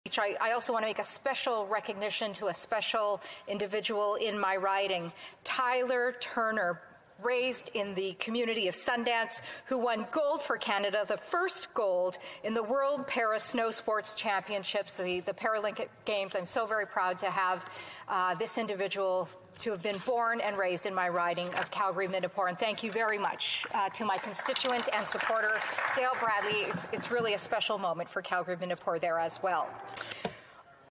Stephanie Kusie congratulates Tyler Turner on his gold medal win!
Tyler Turner of Calgary has won Canada’s first gold medal at the World Para Snow Sports Championships with a victory in the men’s snowboard cross!
A parachuting accident left Tyler paralyzed but he continued on with the support of his family, his spirit for adventure, and courage not to allow his life to be defined by his accident. Nothing can stop this young man!
Congratulations on this amazing achievement, Tyler!